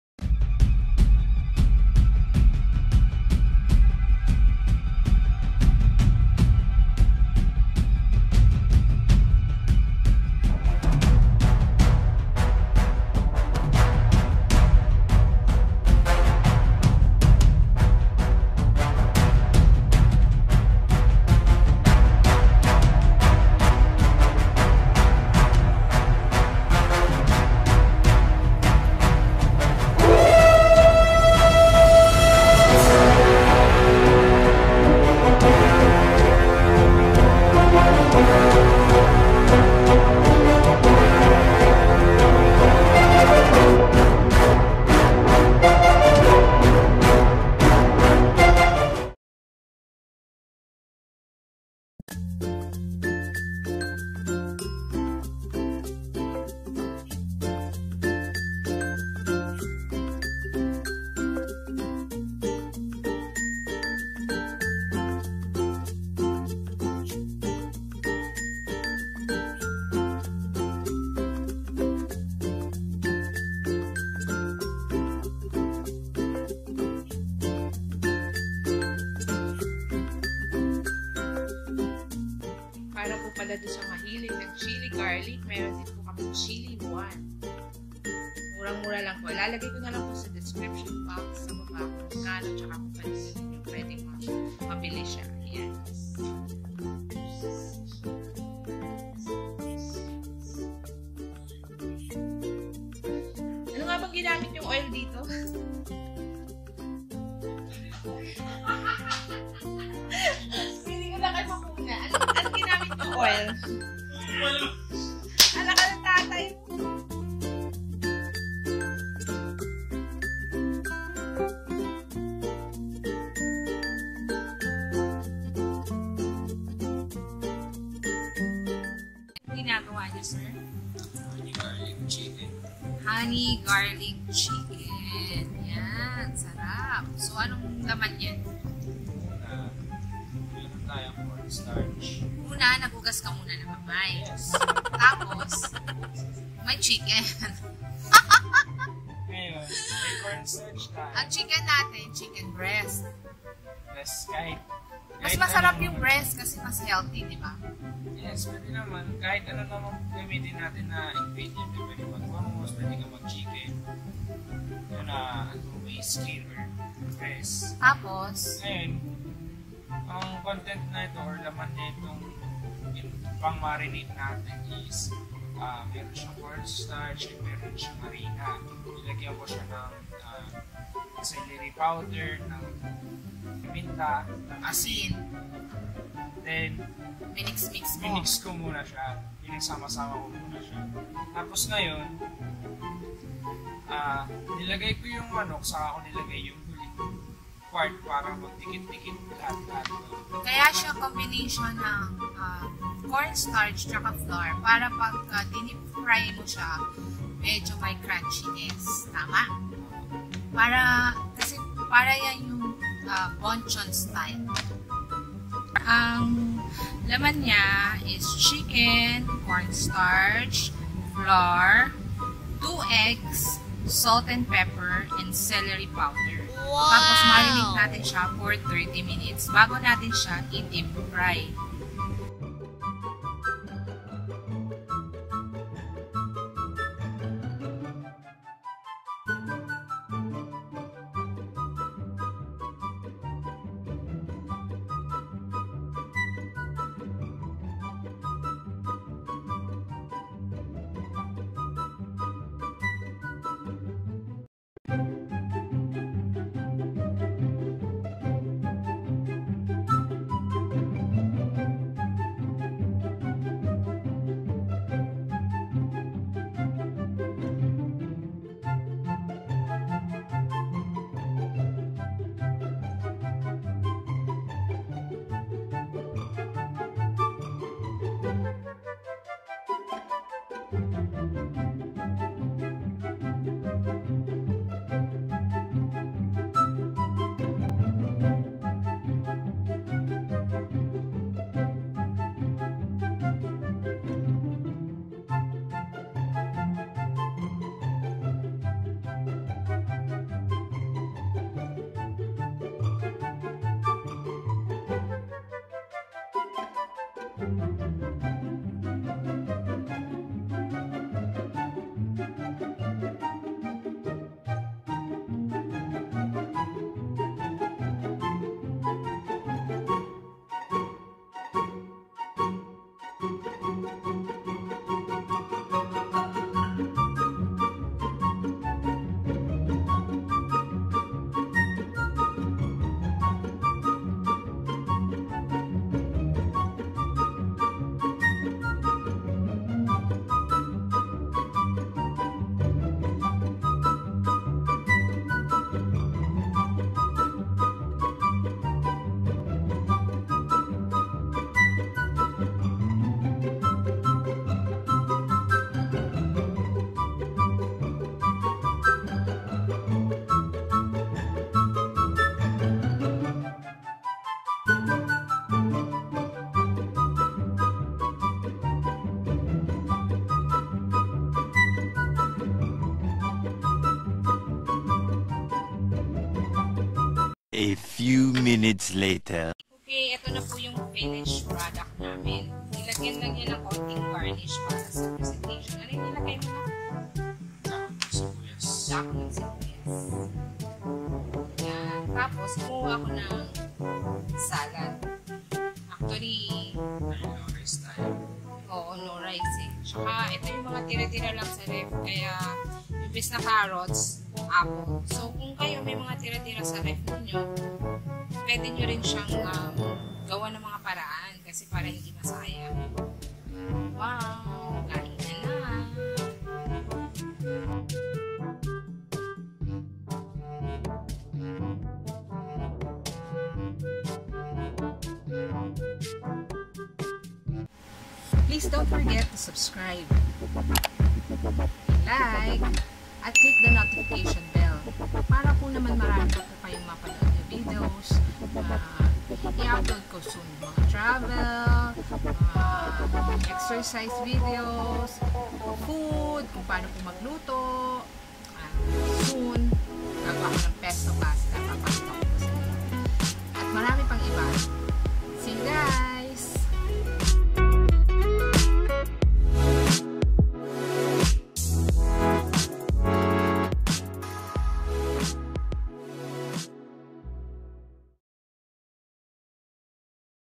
Honey Garlic Chicken ala Mickey
Simple recipe for Honey Garlic Chicken, for those who like it spicy you can add a little Chili garlic sauce.

Ingredients :
1 kl. Chicken Breast
Flour
Cornstarch
2 Eggs
Salt and Pepper
Celery Powder
Catsup
Minced Garlic
Canola Oil
Lee kum kee premium soy sauce
Honey
Water

Kasama Ko ang aking anak na mahilig sa pagluluto,

Salamat po and God Bless